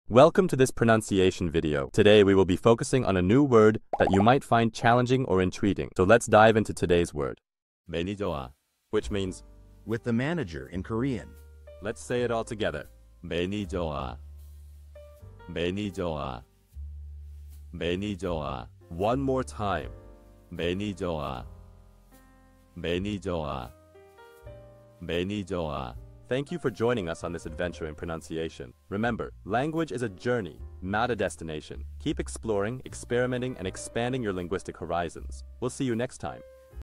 How to pronounce maenijeowa | 매니저와 (With the manager in Korean)
Master the Pronunciation of 'maenijeowa | 매니저와' - which means : With the manager in Korean 📝 with @PronunciationKorean 🎓🗣️ - [Your Guide to Korean]

Learn to pronounce 'maenijeowa' perfectly in Korean with our easy and engaging tutorial by @PronunciationKorean! 📖📽️ In this comprehensive video, we provide you with native speaker examples 🗣️👂 and phonetic breakdowns 🎵 to ensure you get the pronunciation just right.